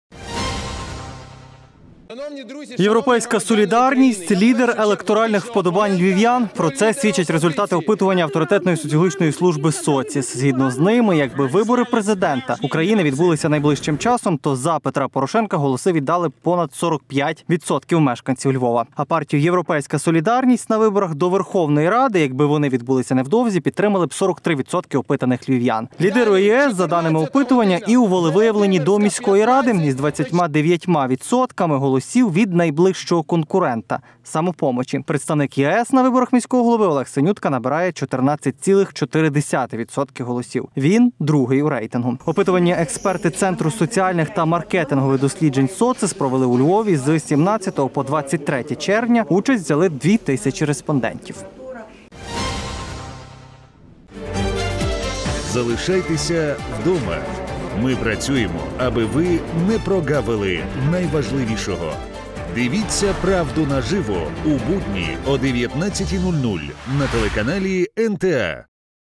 "Європейська солідарність" - лідер електоральних вподобань львів'ян
"Європейська солідарність" - лідер електоральних вподобань львів'ян. Про це свідчать результати опитування авторитетної соціологічної служби «Соціс».
Згідно з ними, якби вибори Президента України відбулися найближчим часом, то за Петра Порошенка голоси віддали б понад 45 % мешканців Львова. А партію «Європейська Солідарність» на виборах до Верховної ради, якби вони відбулися невдовзі, підтримали б 43% опитаних львів’ян. Лідирує "ЄС", за даними опитування, і у волевиявленні до міської ради. Із 29-ма відсотками голосів від найближчого конкурента «Самопомочі». Представник "ЄС" на виборах міського голови - Олег Синютка набирає 14,4 відсотки голосів. Він другий  у рейтингу.

Опитування експерти Центру соціальних та маркетингових досліджень "Социс"провели у Львові з 17-го по 23-тє червня. Участь взяли 2 тисячі респондентів